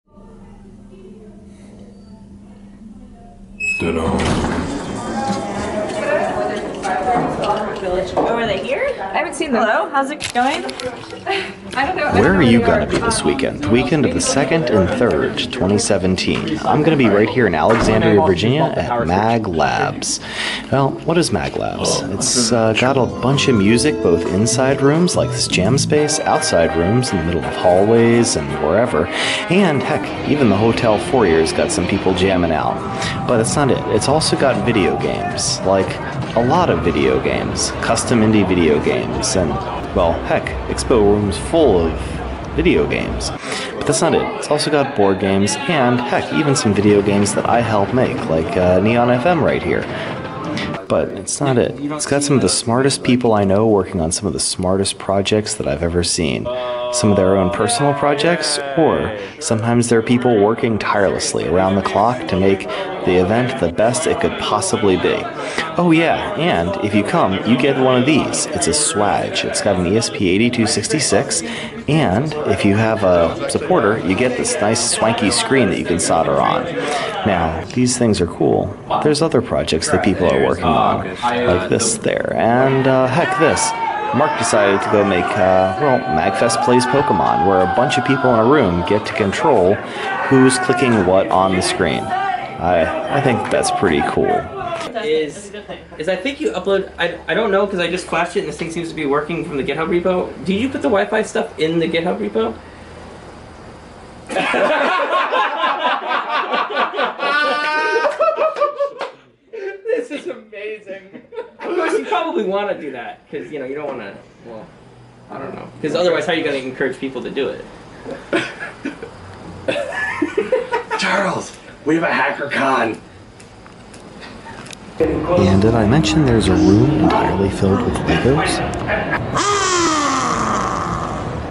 I'm a MAGLabs 2017 (Sept 2,3)
I've been helping out a lot with MAGLabs.  Mostly with the ESP8266-based swadges!  But I'm hoping to hang out!  Come on by!

Swadges/ESP8266 Badges for full-weekend tickets only (not single-day passes)

Thanks to Greg and Sheva for recording most of the footage!  And to the whole research team here at MAGLabs for putting this crazy event on!